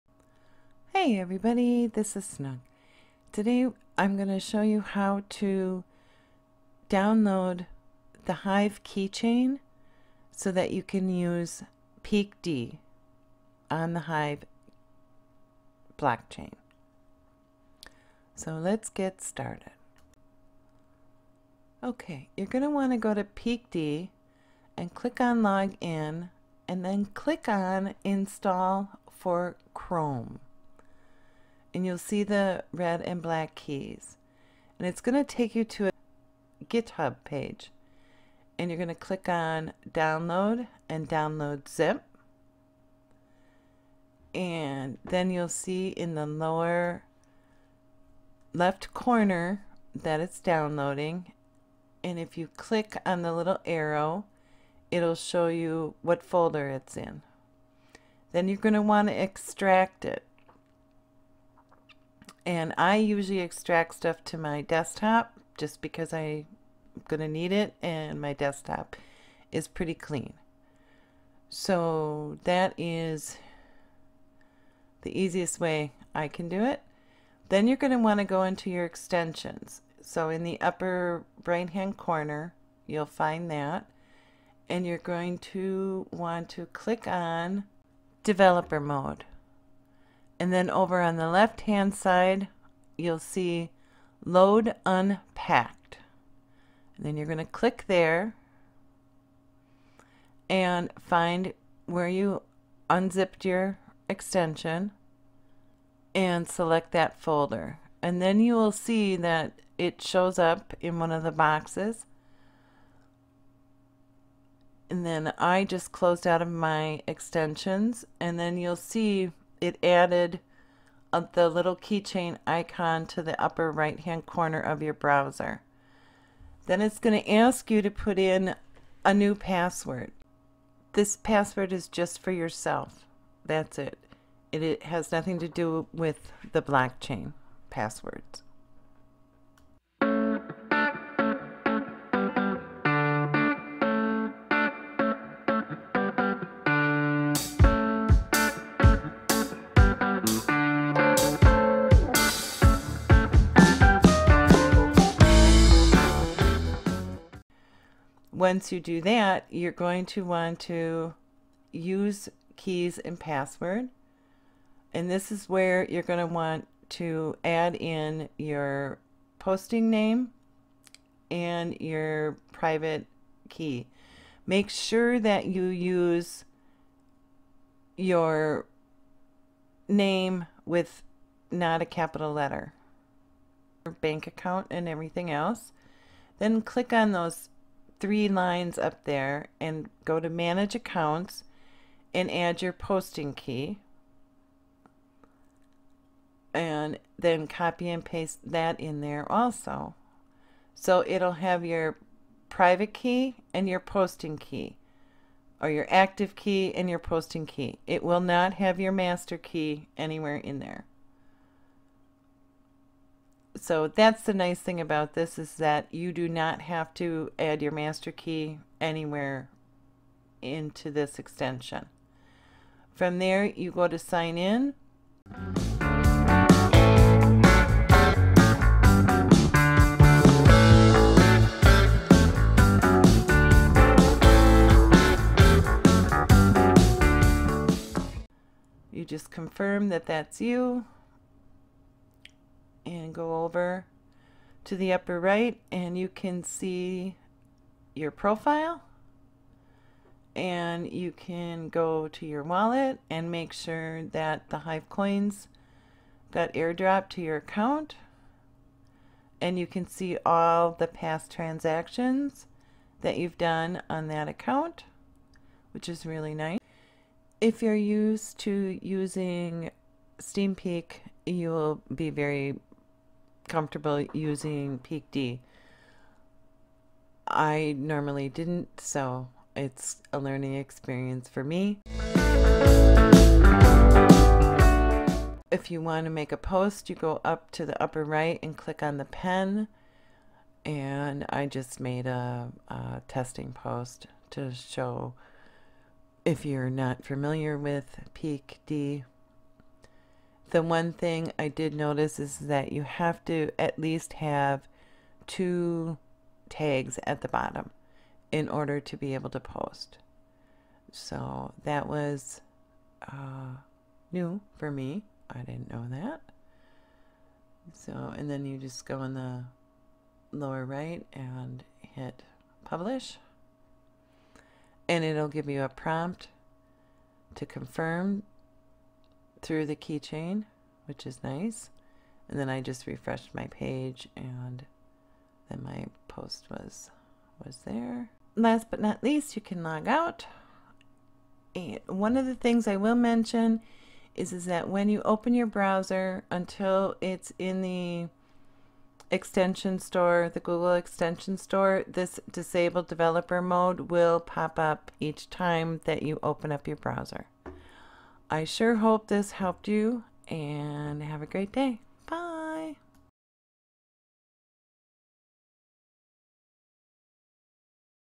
How to add Hive Keychain on Chrome
If you have read the post [Hive Keychain (Beta) is ready] by @stoodkev and are still confused about how to add the Hive Keychain to you Chrome browser the above video should answer all of your questions.